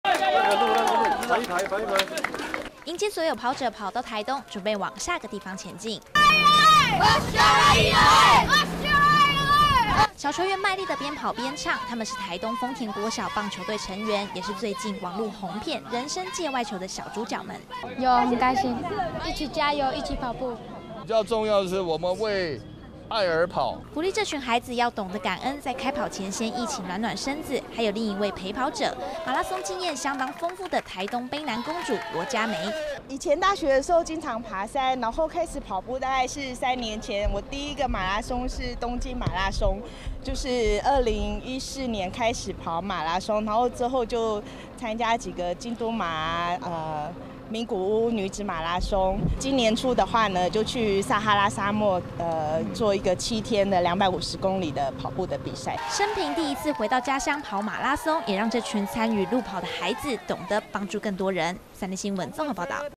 路跑傳愛 豐田少棒球員感恩陪跑｜三立財經台CH88
為響應台灣史上第一次不斷電接力馬拉松，第四站來到台東，跟豐田國小少棒隊球員碰面，陪跑一小段路程，替跑者加油打氣。